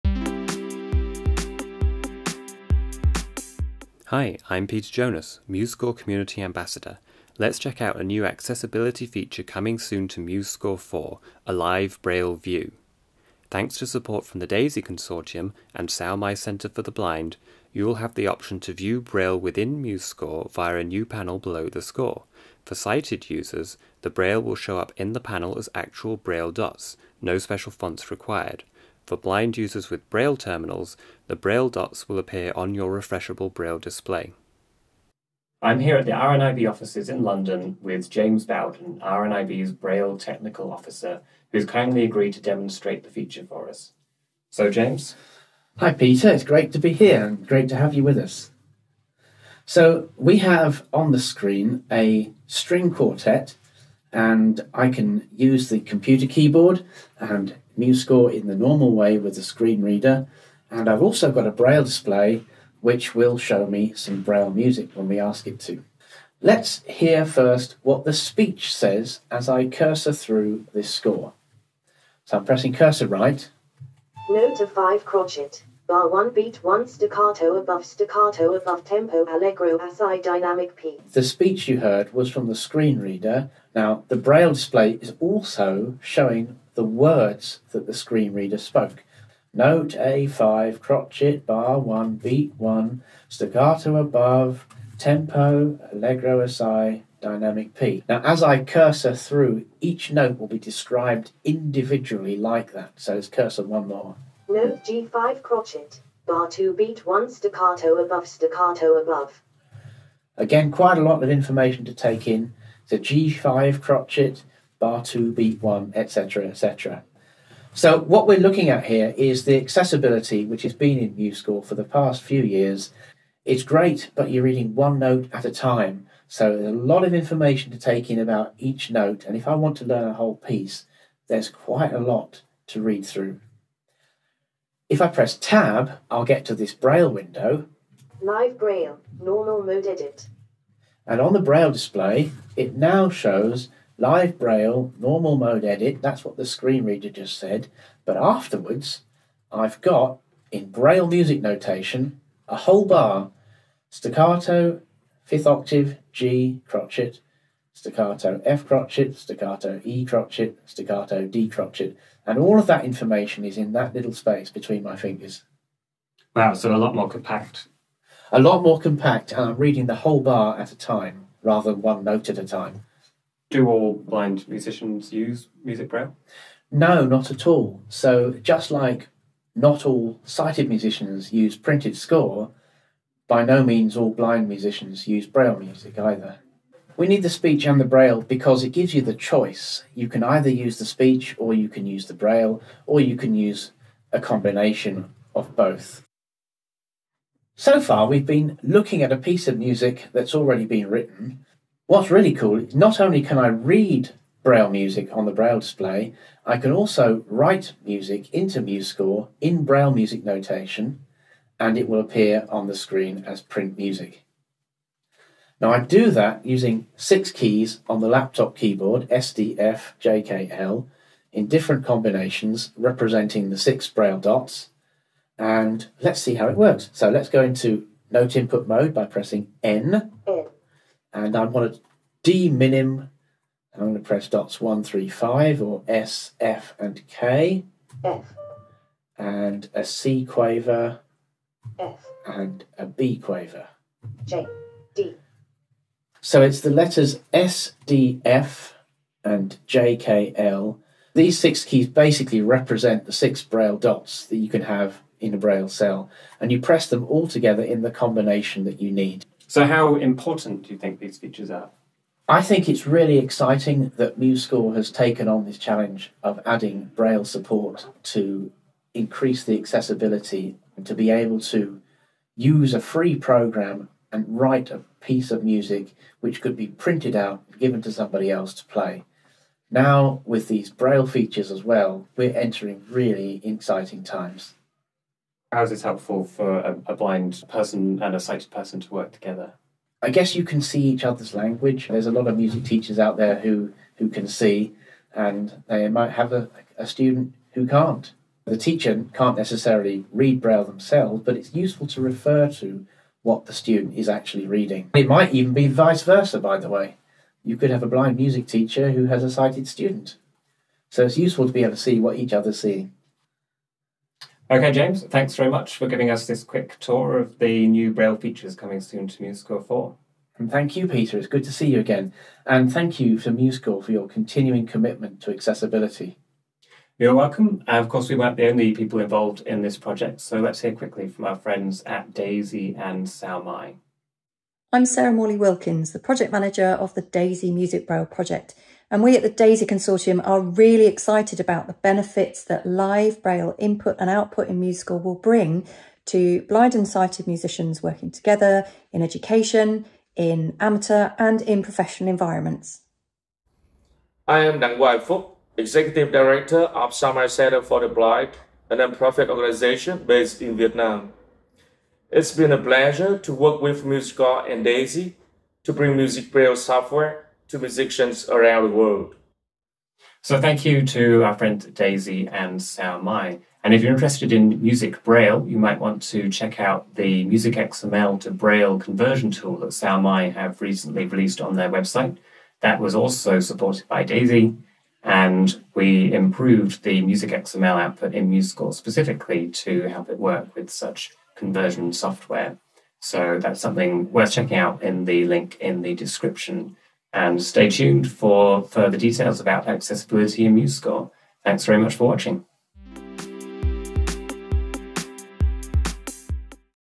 MuseScore 4.1: Live Braille Features
In this film, we demonstrate two groundbreaking accessibility features coming soon to MuseScore 4: a live view of the score in Braille music notation, and Braille input via the computer keyboard. These features will arrive in the upcoming releases of MuseScore 4.1 and 4.2 respectively.

The braille features were developed with support from the DAISY Consortium and Sao Mai Center for the Blind. As mentioned in the video, the DAISY Consortium also supported development of MuseScore 4’s MusicXML exporter, and Sao Mai’s free online MusicXML to Braille conversion tool that you can try out for yourself

CONTENTS:
0:00 Introduction
0:35 James Bowden, RNIB
0:53 Screen reader demo
2:14 Braille view (new feature)
2:53 About braille music notation
3:17 Braille input (new feature)
4:19 Importance of braille features
5:13 Thank-yous
5:27 DAISY and Sao Mai
6:10 MusicXML and Braille
6:37 Outro